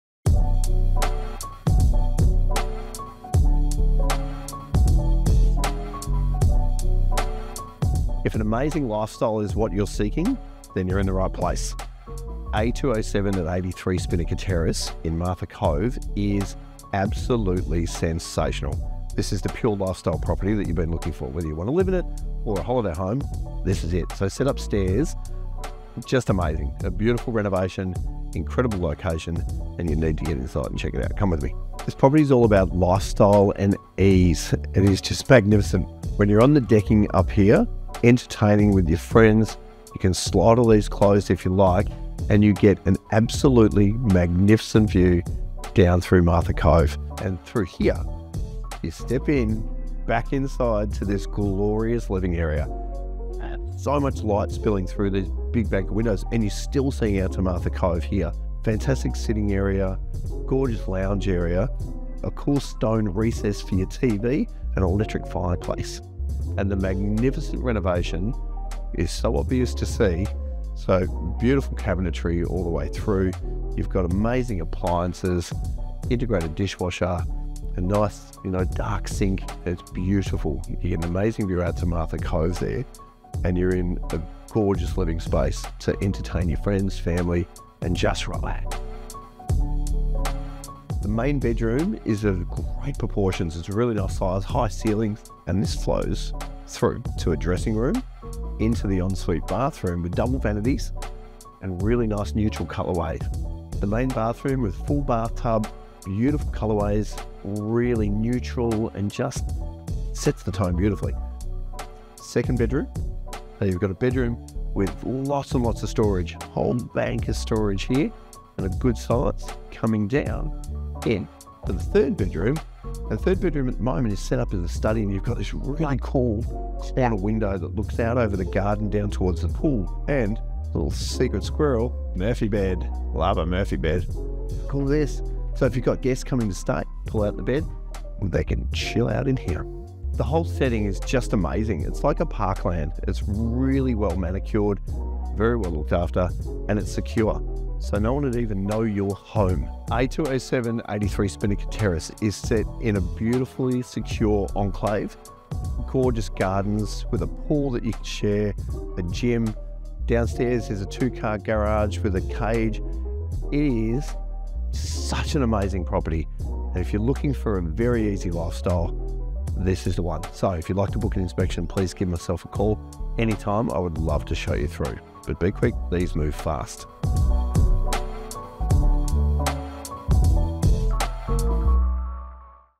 A207 83 Spinnaker Terrace Safety Beach
Waterfront luxury overflowing with class & comfort

Among the most beautifully designed and thoughtfully finished apartments in this luxury waterfront complex near the mouth of Martha Cove, this three-bedroom residence truly embraces seaside living with flexible spaces for lounging and spoiling your guests.

Enjoying a magical outlook to the yachts that glide by on their way out to the bay and with rolling hilltops of Red Hill and Main Ridge in the background, the home offers a privileged lifestyle in this secure seaside complex surrounded by beautiful gardens, a heated inground pool, a gymnasium and with a gate access directly onto the marina's waterside board walk leading to the beach.

Inside has been recently refurbished with brilliant additions from the toasty flame of the electric fireplace to the enhanced stone-topped kitchen with extra cupboards, extra bench space and beautiful new decor, while the large third bedroom has a pull-out queen-size Murphy bed, built-in desk and wall of robes allowing you to use it for living space, a home office or as a guest room.

On either side of the kitchen are separate lounge and dining areas, while the covered alfresco area directly overlooking the water provides wonderful space for your bar fridge and BBQ, with clear pull-down blinds and auto awnings for all weather use.

The main bedroom has a walk-through dressing area with mirrored robes and a ensuite with twin vanity and extensive mirrors for a sense of extra space, while down a hallway at the other end of the home two other bedrooms with full robes use the family bathroom with shower and deep bath.

The home comes complete with a full laundry, ducted reverse-cycle heating and refrigerated cooling as well as remote-controlled access to basement parking for two cars and storage area.

An easy walk to the vibrant boat harbour and Martha's Table fine dining restaurant, or in the other direction to the beach and sailing club for a lovely meal or drinks on the clubhouse deck at sunset, a beachside path and boardwalk provides a lovely journey along the stretch of beach to Dromana town centre.